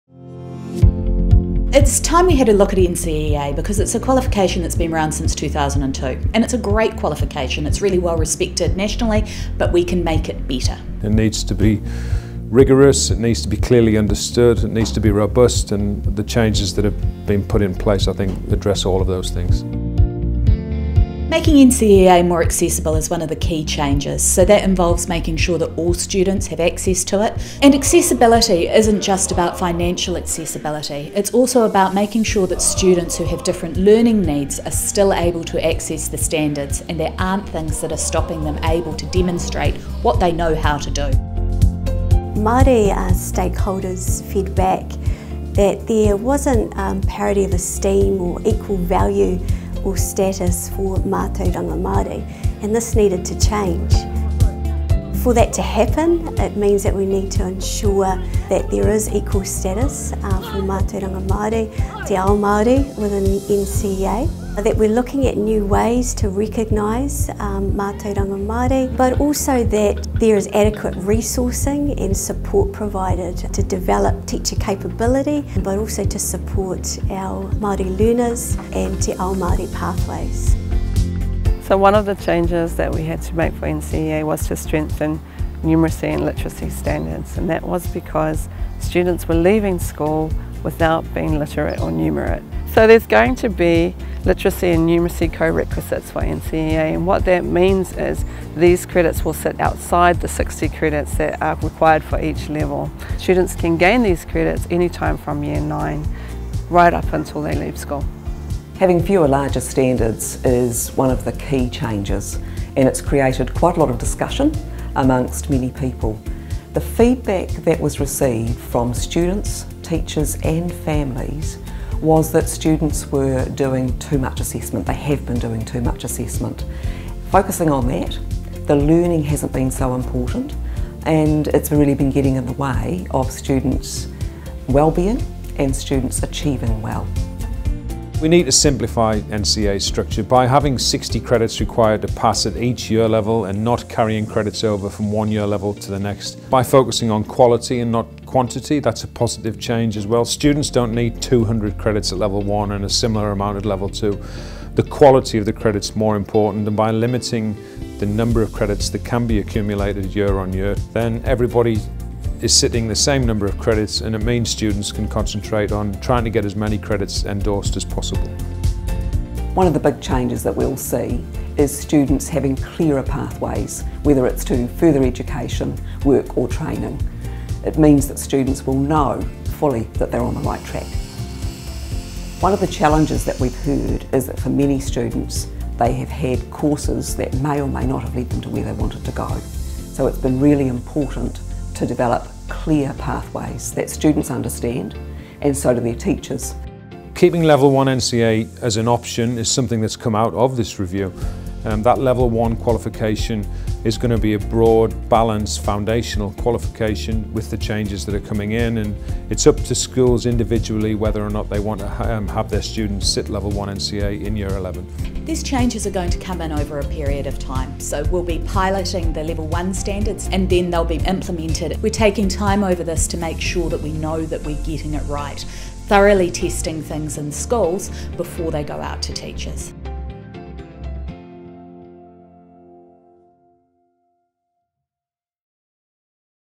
Changes to strengthen NCEA
We are working with schools and communities to strengthen NCEA so our young people have a solid foundation to be successful in life and in a global economy.